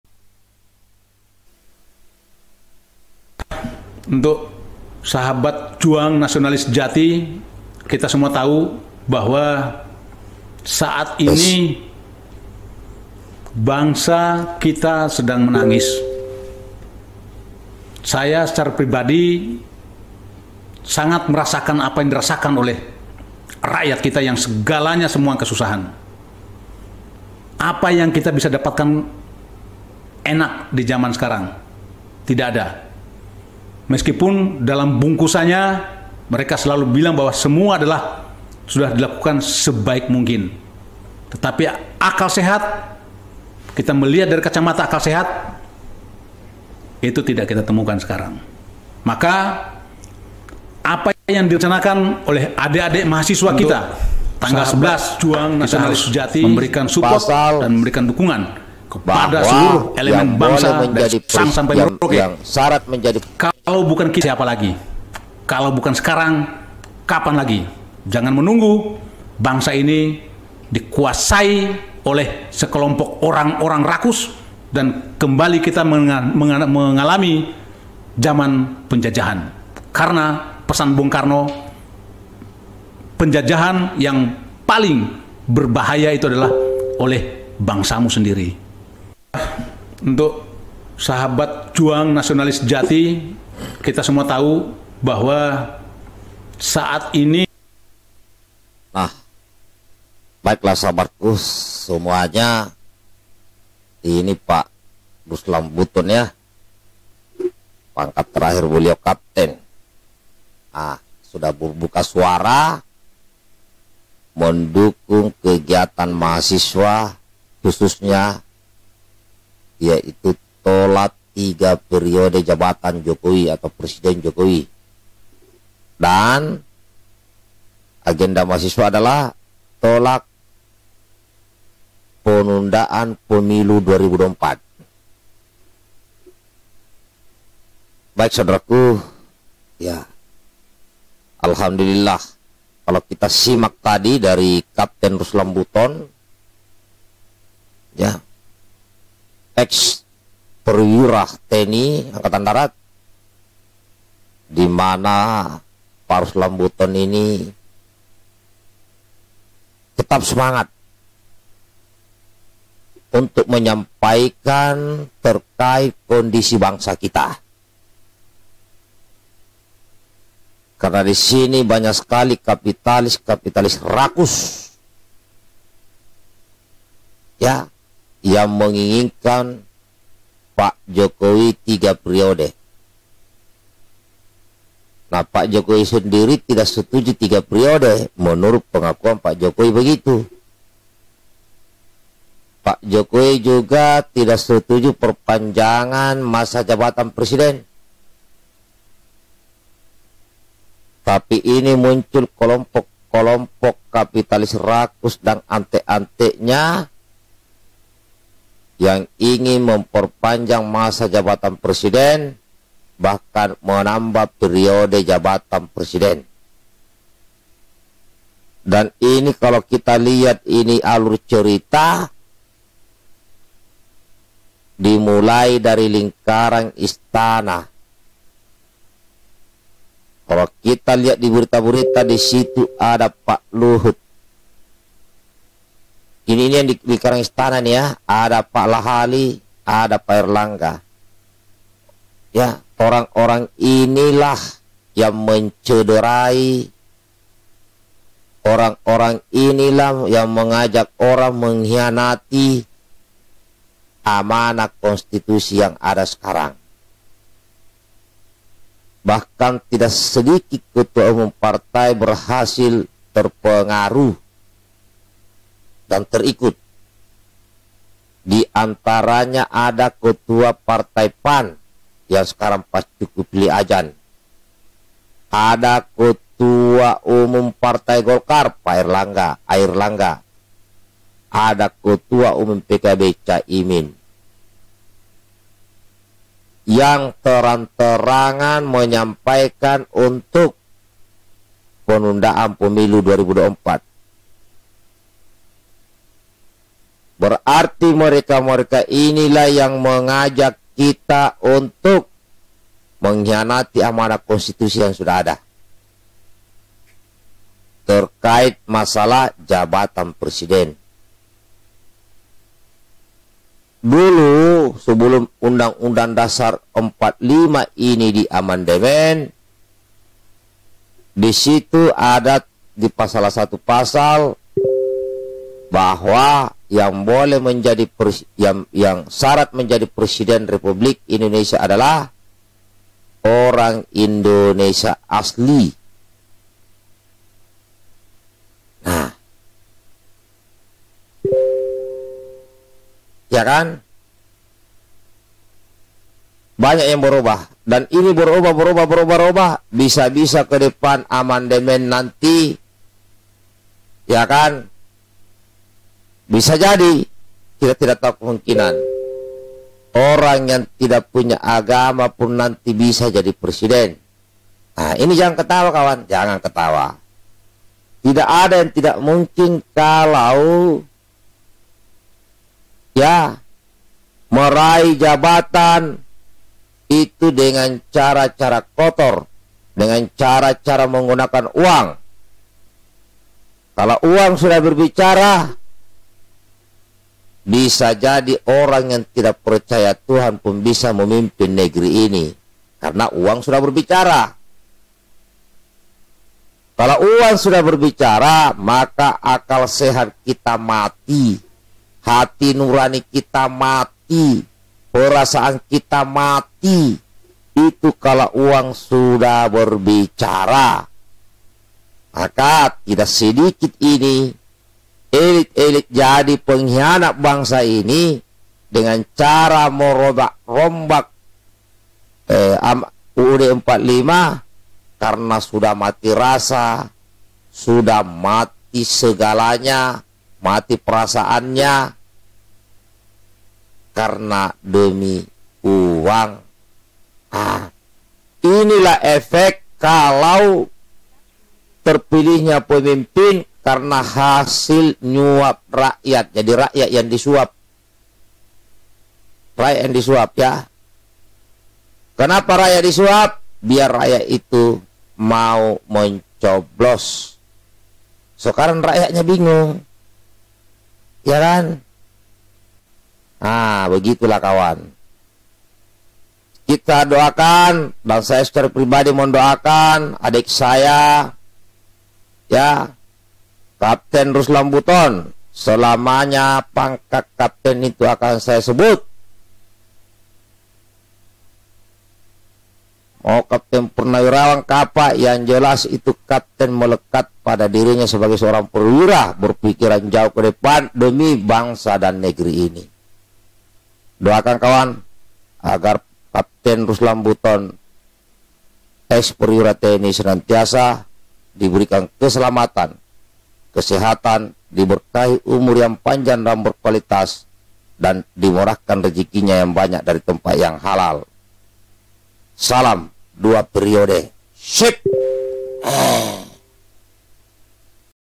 Lama Tak Muncul. Sekali Muncul Langsung GEGER, Kapten Ruslan Buton Dukung Aksi Mahasiswa.MPS: MANTAP
Aamiin.

TTD